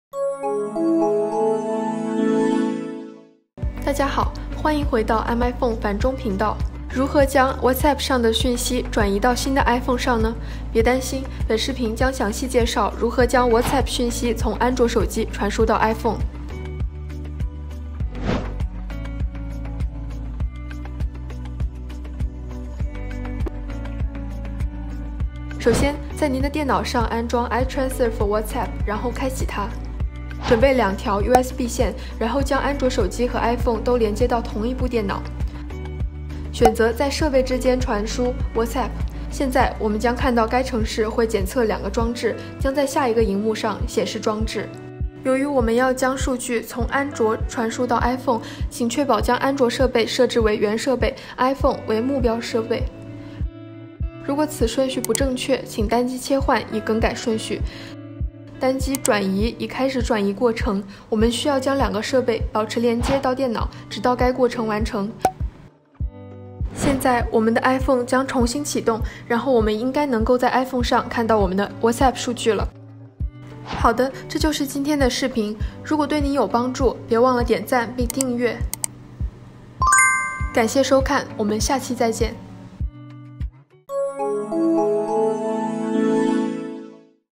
將 WhatsApp 從 Android 轉移到 iPhone -- 不用藉助 Google Drive 和 iCloud 將 WhatsApp 轉移到新手機！
🤔最近是否不再使用 Android 手機，改換 iPhone 新機？並需要傳輸 WhatsApp 訊息和媒體檔案？在影片中，我們將使用最佳 WhatsApp 轉移工具 iTransor for WhatsApp 。快來試試吧！

👉影片內容：
0:28​ 下載WhatsApp 轉移工具
0:33 連接Android 和 iPhone 手機到電腦
0:59 開始轉移數據